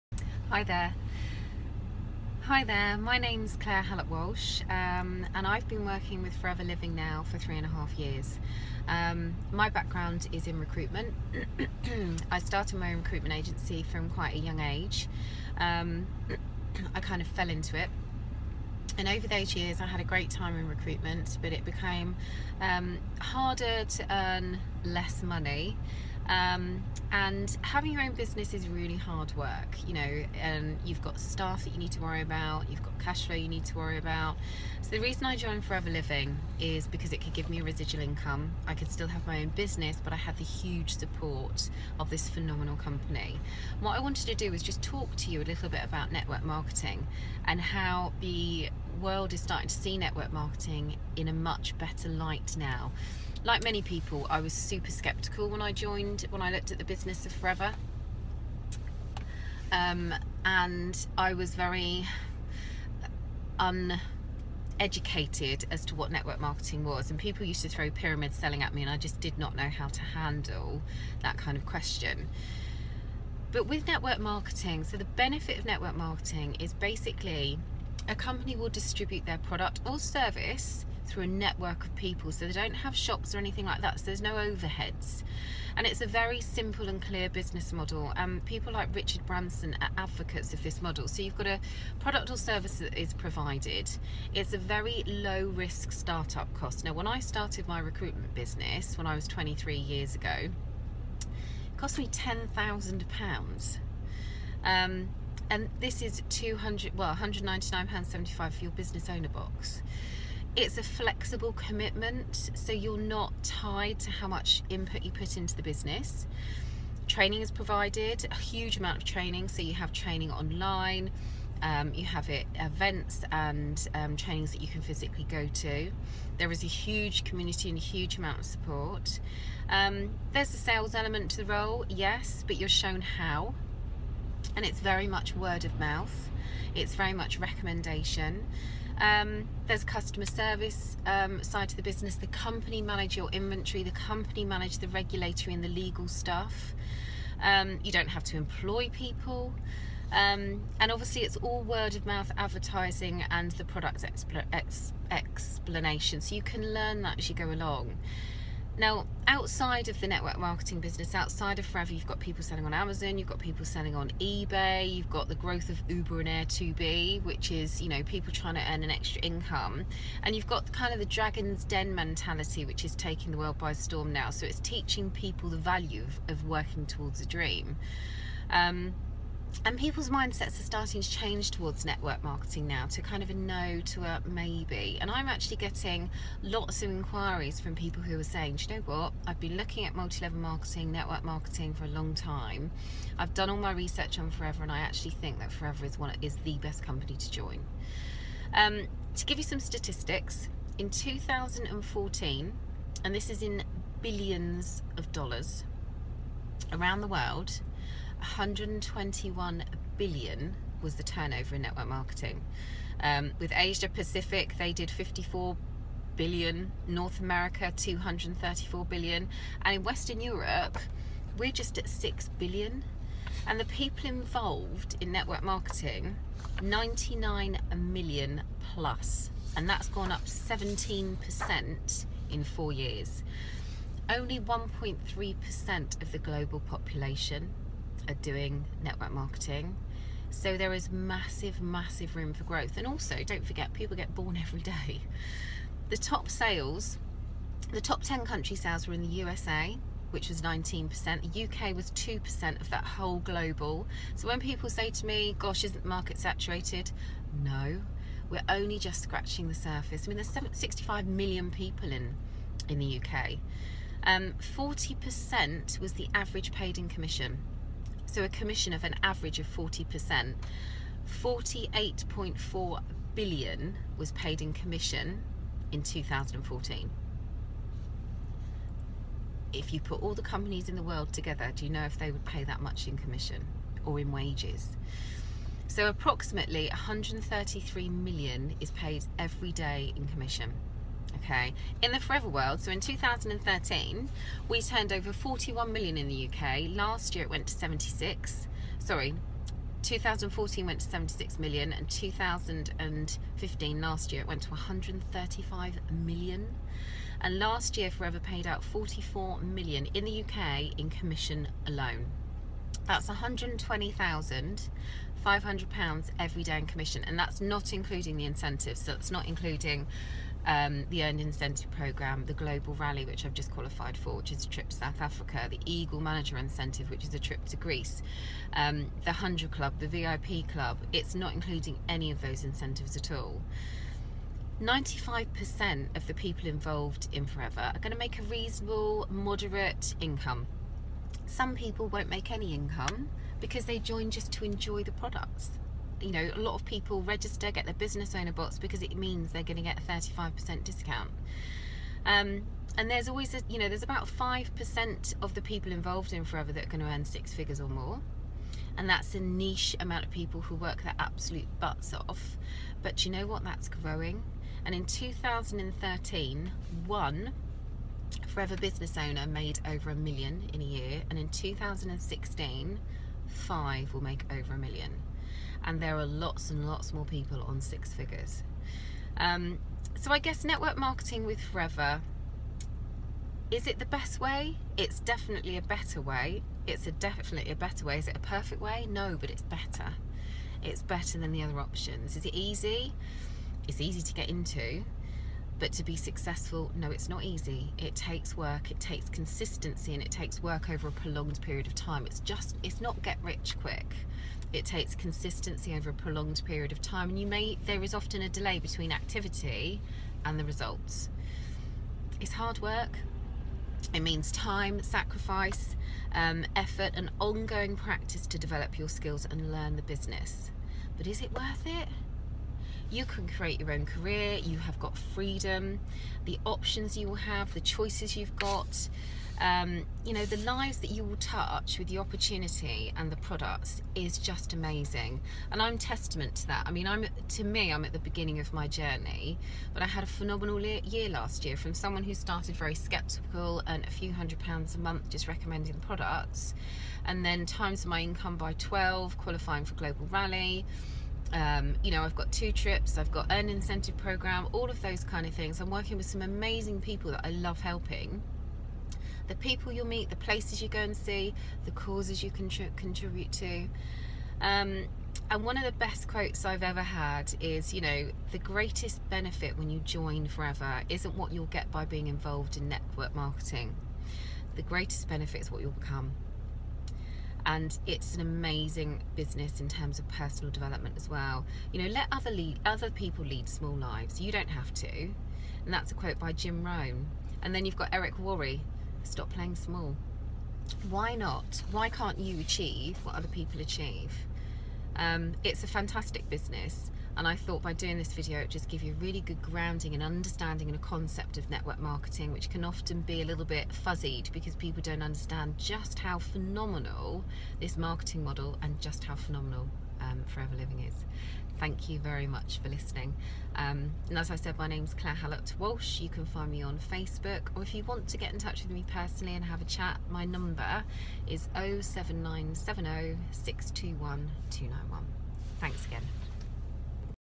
Why MLM? Why Forever Living?
Facts and figures about Multi Level Marketing and how the business model is becoming more and more popular, and why Forever is the number one company in the industry.